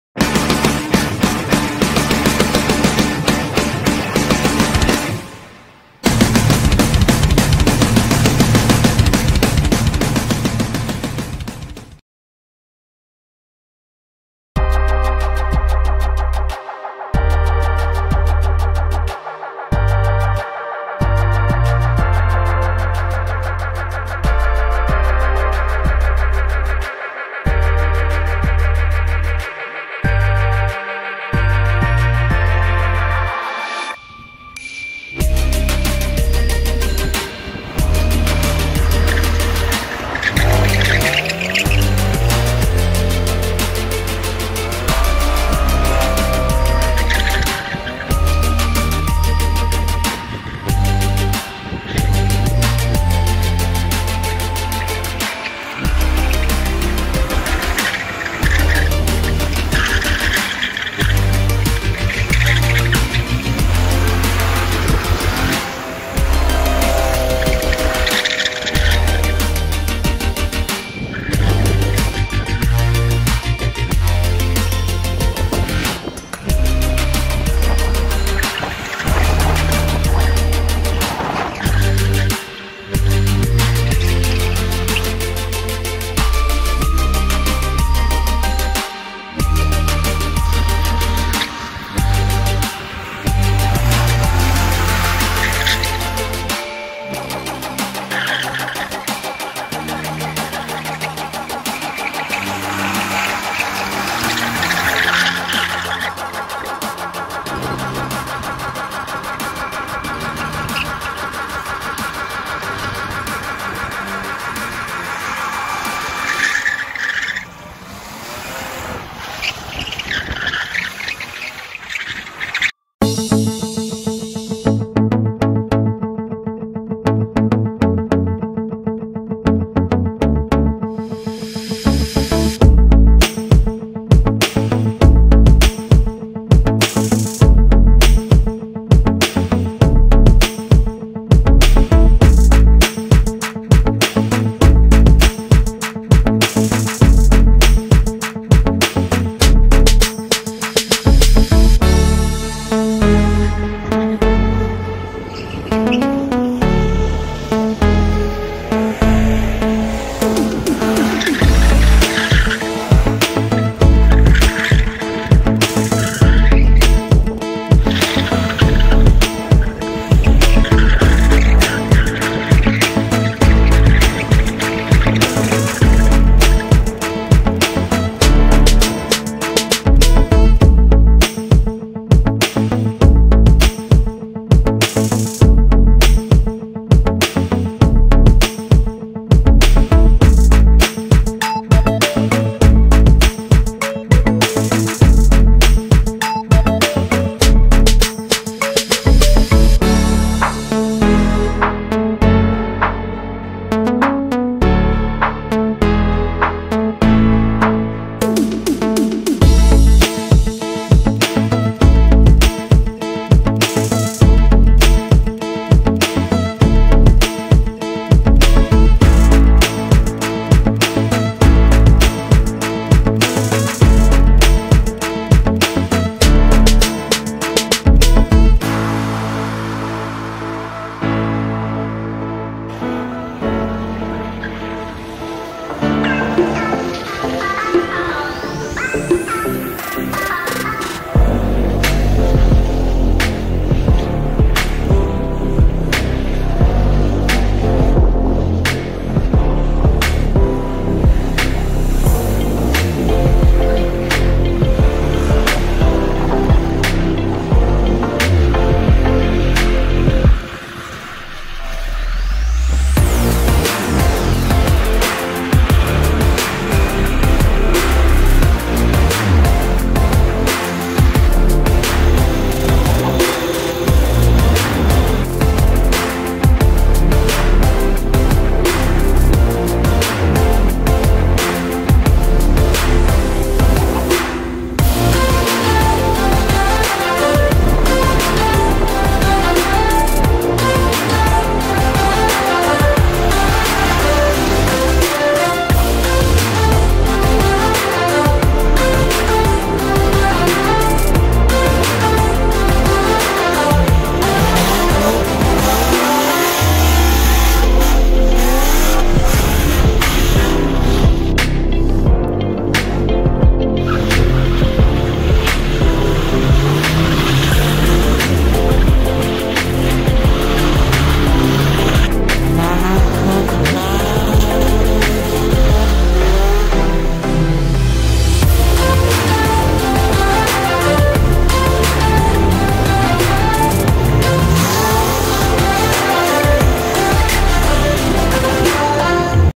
Dobrzyca DRIFT #11
Dzisiaj przy lekkich opadach deszczu nawierzchnia sprzyjała lataniu bokiem ale obowiązki sprawiły, że nieliczni mogli się bawić. Fajnie, że mimo silnego wiatru udało się zrobić kilka ujęć z drona, które zawsze ciekawie urozmaicają filmik.
Jeżeli spodobał ci się ten materiał to nie zapomnij zostawić suba i lajka.